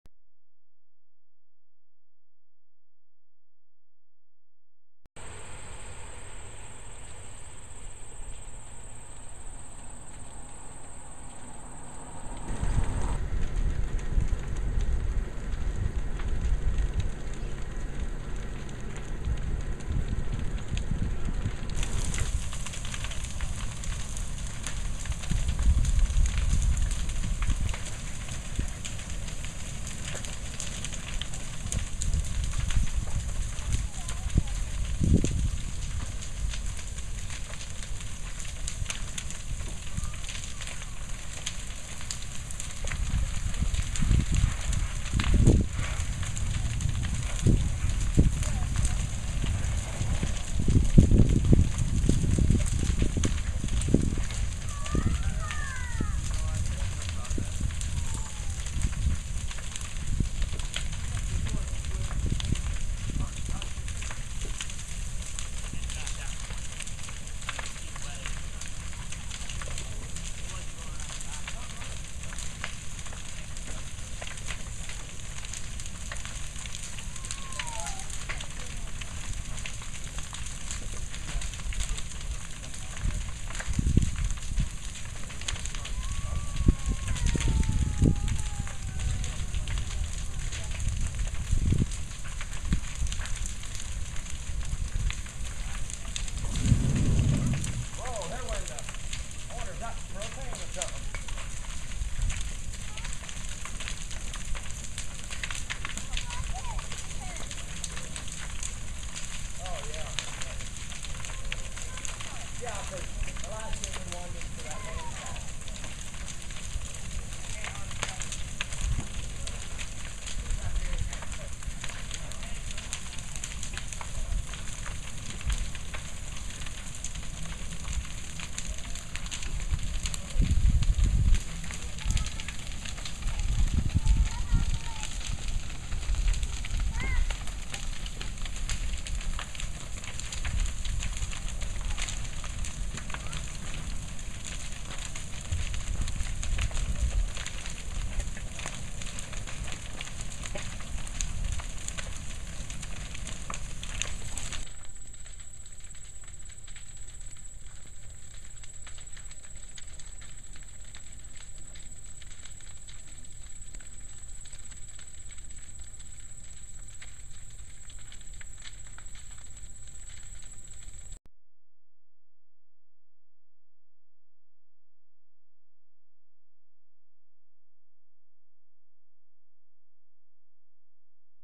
Fire in the Country
While returning from an auction I spotted this black smoke pouring into the sky. I found out these young fellows were trying to burn down an old corn crib. Unfortunately a propane tank and a new barn were very close. After some hosing down with water everything turned out ok.